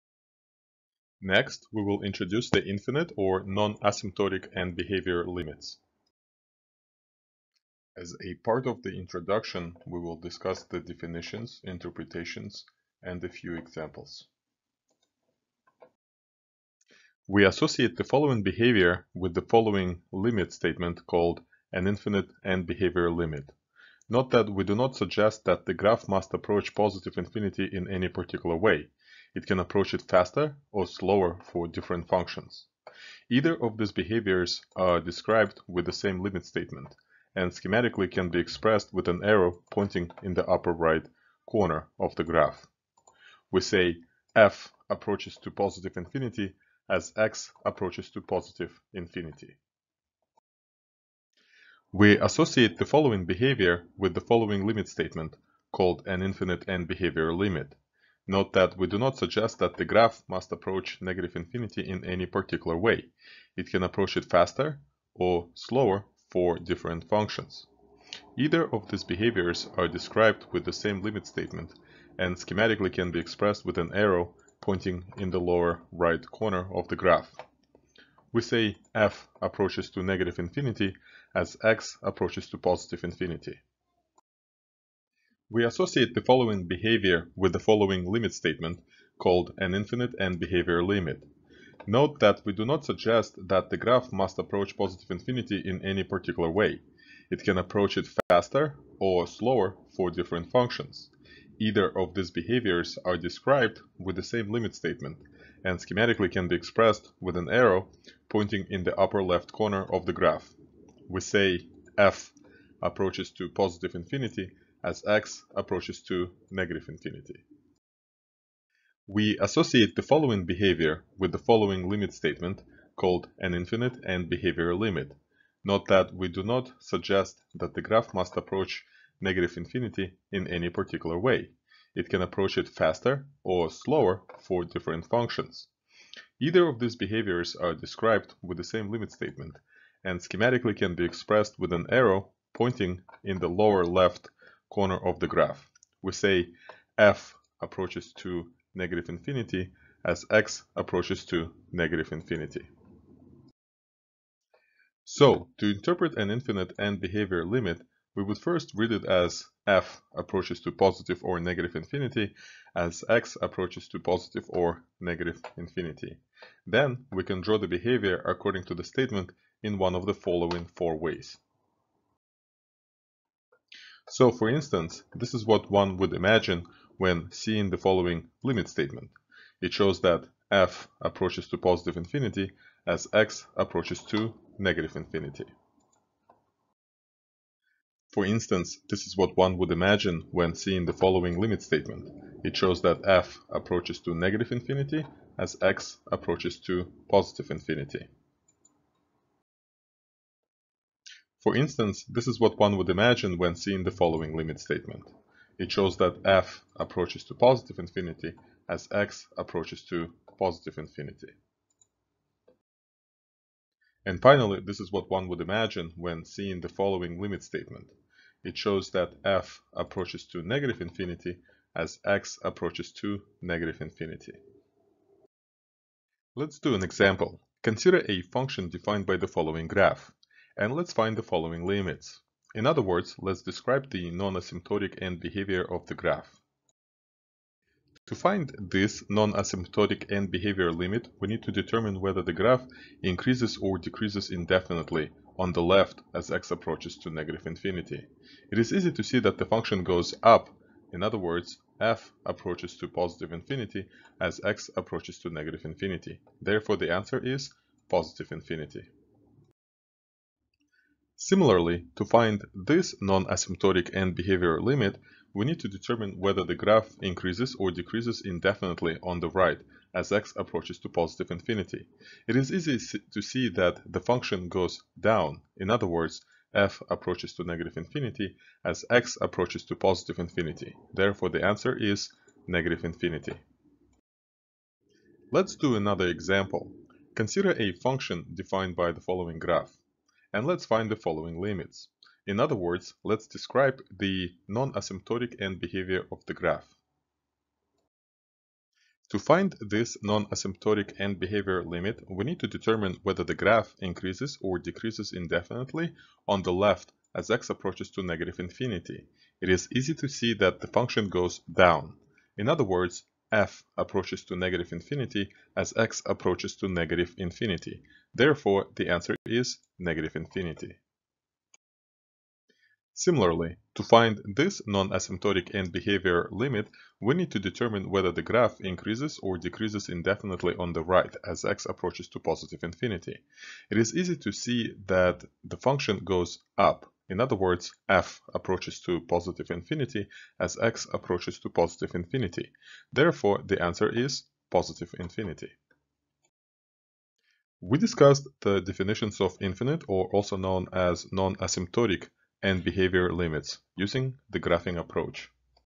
Infinite (Non-Asymptotic) End-Behavior Limits
In this video, I introduce infinite (non-asymptotic) end-behavior limits using the graphing approach. To navigate the lecture, you may use the following timecodes:
0:00 Intro
0:16 Definition of an infinite (positive) right-end-behavior limit
0:51 Definition of an infinite (negative) right-end-behavior limit
1:25 Definition of an infinite (positive) left-end-behavior limit
1:58 Definition of an infinite (negative) left-end-behavior limit
2:34 Interpretation of an infinite (non-asymptotic) end-behavior limit
3:45 Example of an infinite (non-asymptotic) end-behavior limit
4:44 Example of an infinite (non-asymptotic) end-behavior limit
5:47 Outro